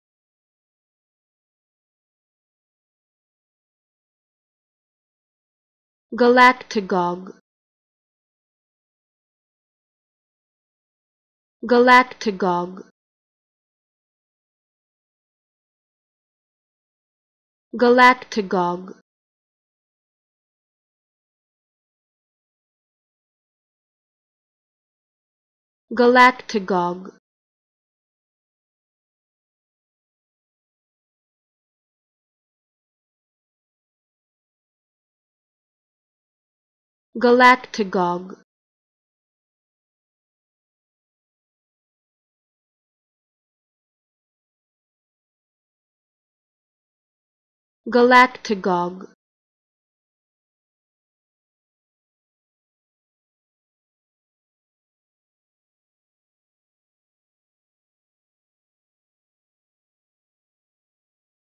How To Say Galactagogue
Learn how to say Galactagogue with EmmaSaying free pronunciation tutorials.